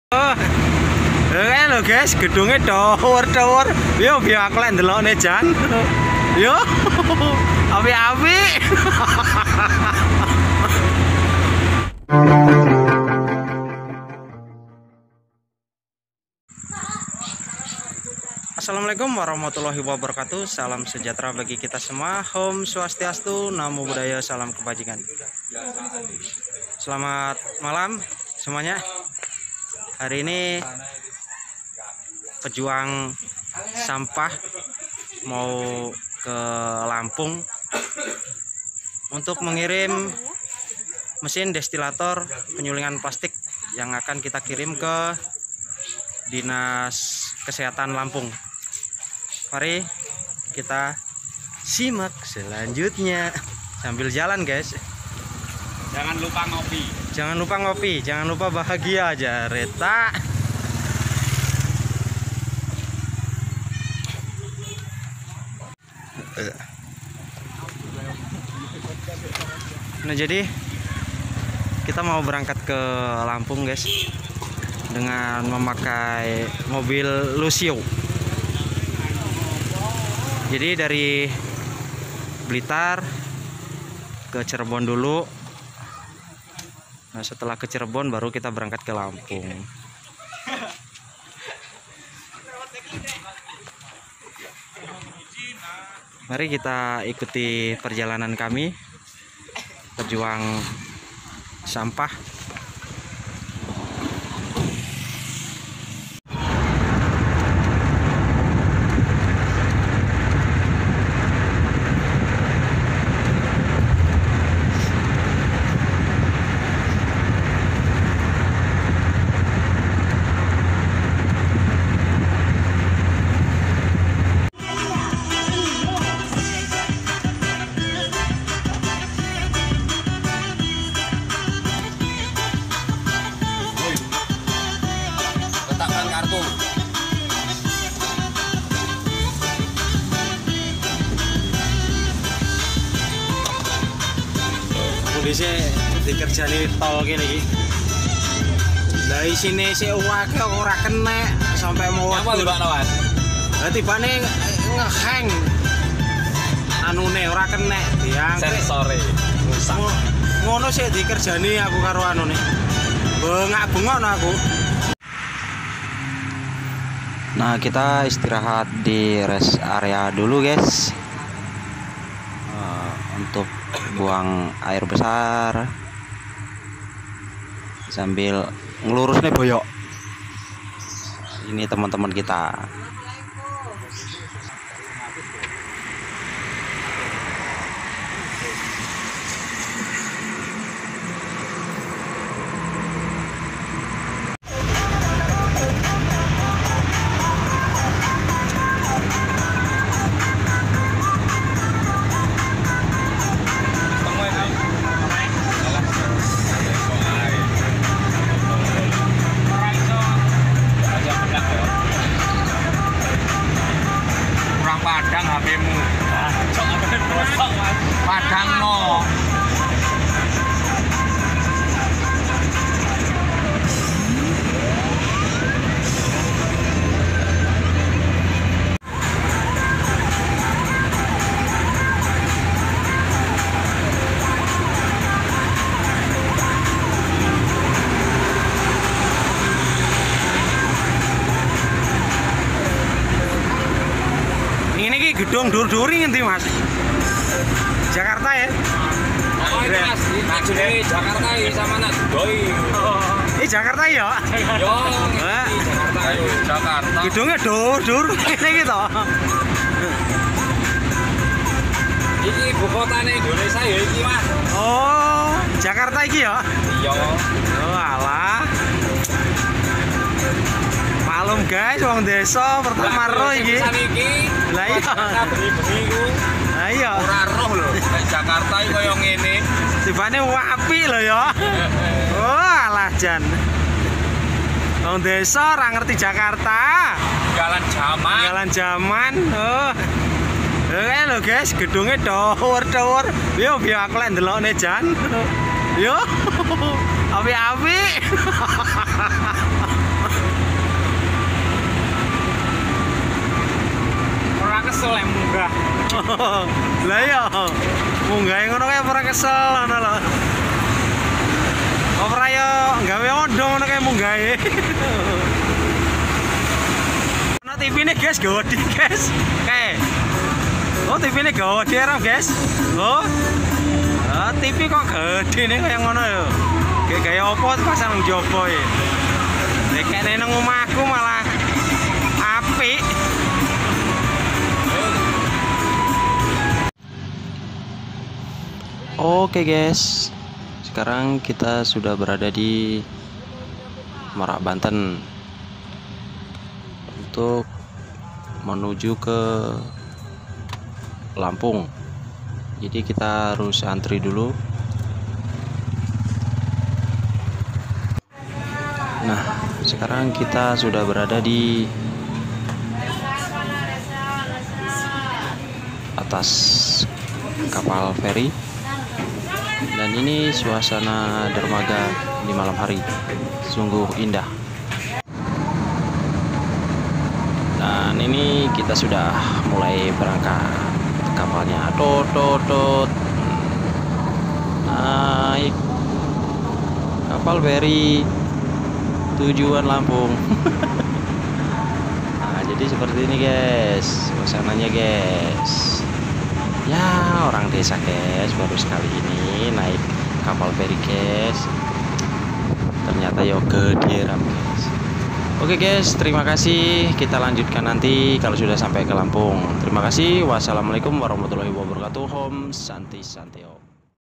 Perjalanan menuju lampung || Sri katon || Didi3Rwlingi
Malam jadi Siang Siang jadi Malam Hari ini Kami Dalam Perjalanan menuju lampung selatan untuk pengiriman mesin destilator sampah plastik yang Di pesan dinas kesehata lampung semoga ini menjadi motifasi masarakat lampung untuk mengolah sampah plastik menjadi bbm,